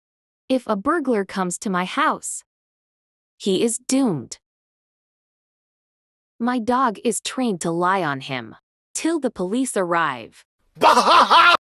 If a BURGLAR Comes To My House 🐶 Cute Maltese Dog 🐶 Funny Dog Videos Shorts
My dog is trained for burglar 🐶 Funny Maltese dog videos🐶 Cute Maltese puppy shorts🐶 Bichon maltese